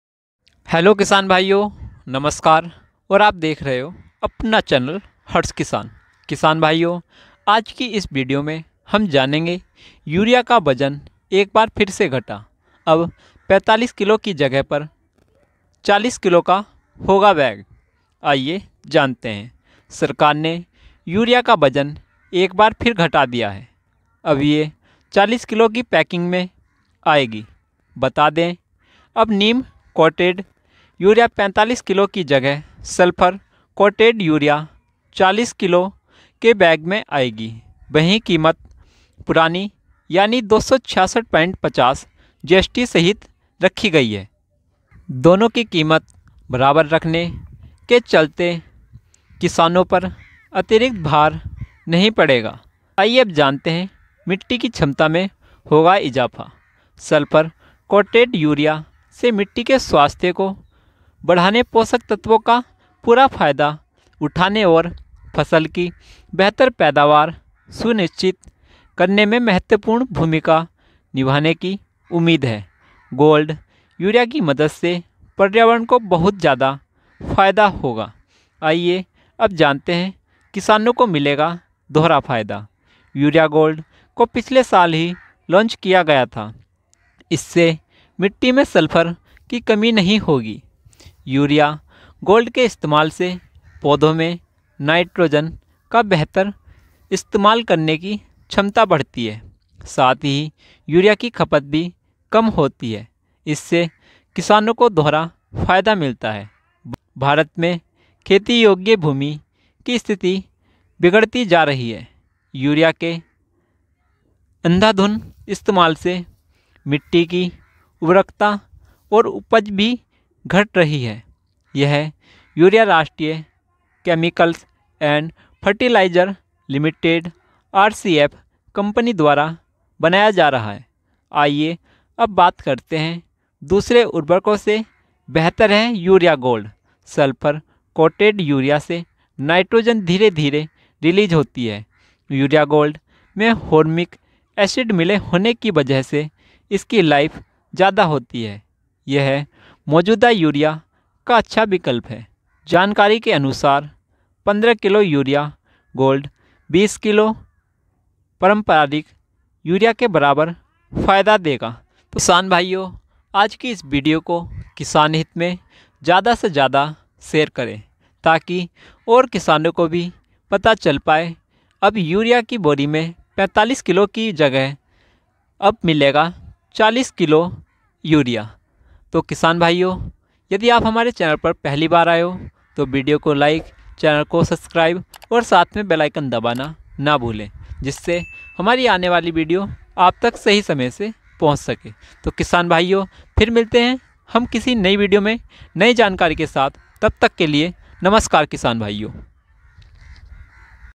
अब 45 की जगह 40 किलो की ही होगी यूरिया की बोरीबोरी || खाद का नया रेट || Fertilizer Prices
सरकार ने यूरिया का वजन एक बार फिर घटा दिया है। अब ये 40 किलो की पैकिंग में आएगी। बता दें अब नीम कोटेड यूरिया 45 किलो की जगह सल्फर कोटेड यूरिया 40 किलो के बैग में आएगी। वहीं कीमत पुरानी यानि 266.50 (GST सहित) ही रखी गई है।